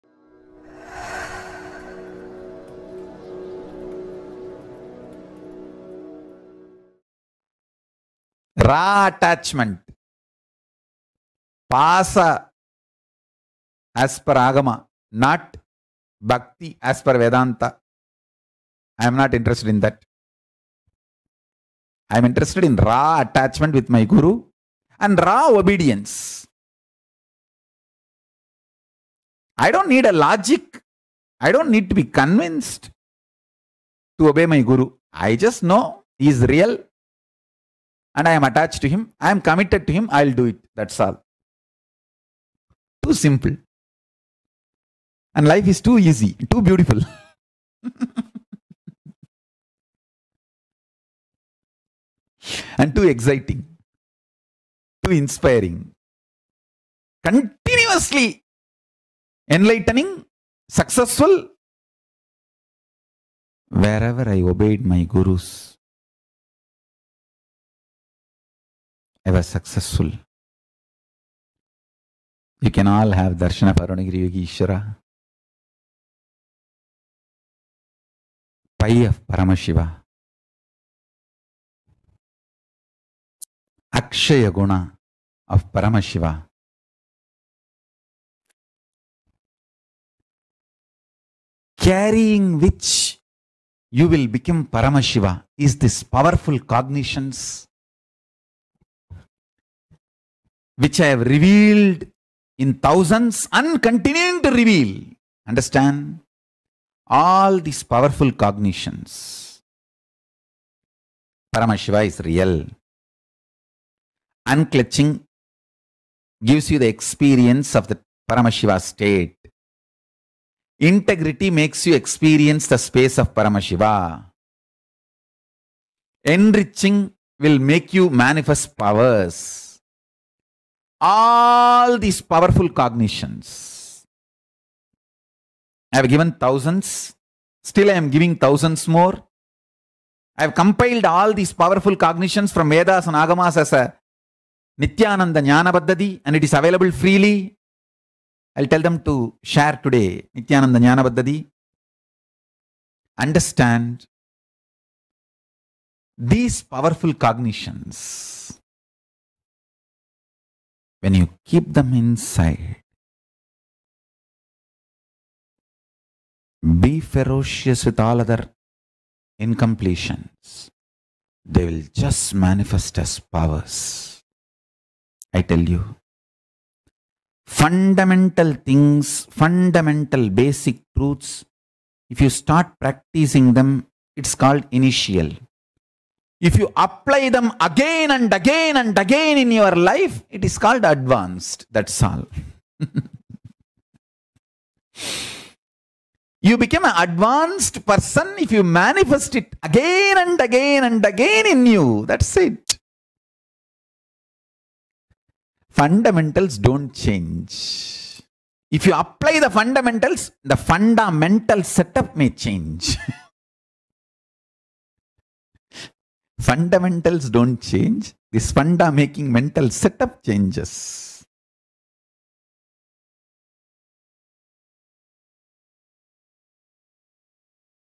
Pasa as Per Agama Not Bhakti as per Vedanta
Raw attachment, Pasa as per Agama not Bhakti as per Vedanta, I am not interested in that, I am interested in raw attachment with my Guru and raw obedience. I don’t need a logic, I don’t need to be convinced to obey my guru. I just know He is real and I am attached to Him, I’m committed to Him, I’ll do it, that’s all. Too simple, and Life is too easy and too beautiful, and too exciting, too inspiring, continuously enlightening, successful. Whereever I obeyed my Gurus, I was successful. We can all have Darshan by Arunagiriyogiswara . PI of Paramashiva, Akshaya Guna of Paramashiva.
Carrying which you will become PARAMASHIVA is this powerful cognitions which I have revealed in thousands, and continuing to reveal, understand, all these powerful cognitions:
Paramashiva is real, unclutching gives you the experience of the Paramashiva’s state, integrity makes you experience the space of Paramashiva, enriching will make you manifest powers, all these powerful cognitions, I have given thousands, still I am giving thousands more, I have compelled all these powerful cognitions from Vedas, and Agamas as a Nithyananda Nyamba  Hamdati, and it is available freely, I’ll tell them to share to-day Nithyananda Nyamba Hamdati.
Understand, these powerful cognitions, when you keep them inside, be ferocious with all of their incompletions, they will just manifest as powers. I tell you, fundamental things, fundamental basic truths, if you start practicing them, it’s called « initial », if you apply them again and again, and again in your life, it is called « advanced », that’s all! You become an advanced person if you manifest it again and again and again in you, that’s it! Fundamentals don’t change. If you applied the fundamentals, the funda mental set up may change.
Fundamentals don’t change, this funda making mental set up changes.